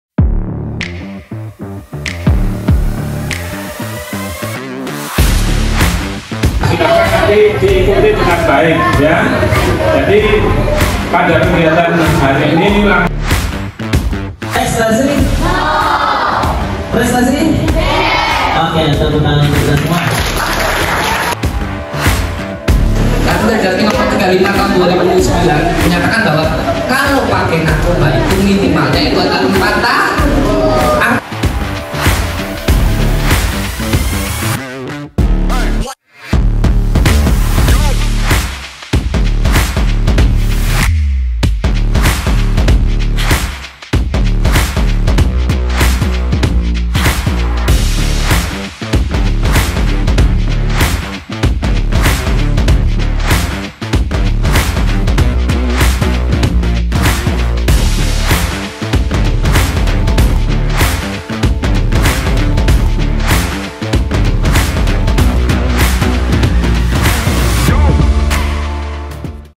Sosialisasi Bahaya Narkoba dan Deteksi Dini Melalui Test Urine Bagi siswa dan Guru SMPN 1 Bati-Bati
Sosialisasi Bahaya Narkoba dan Deteksi Dini Melalui Test Urine Bagi siswa dan Guru UPTD SMPN 1 Bati-Bati